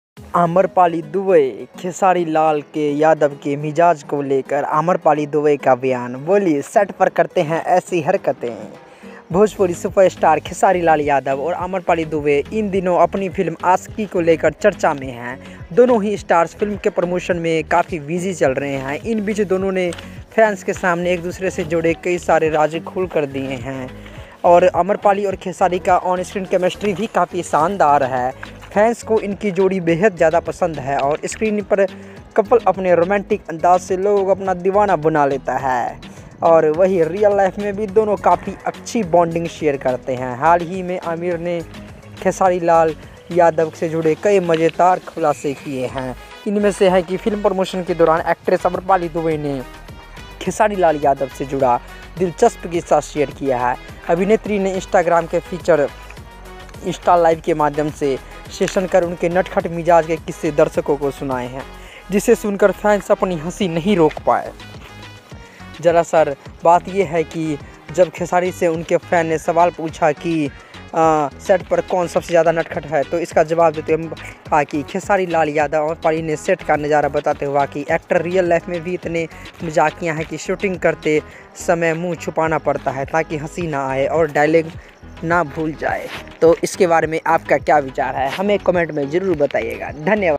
खेसारी लाल यादव के मिजाज को लेकर आम्रपाली दुबे का बयान, बोलीं- सेट पर करते हैं ऐसी हरकतें
your queries;

in this video I describe about the khesarilalyadav new movie shoot with amarpali dubey and discussable moments of this movie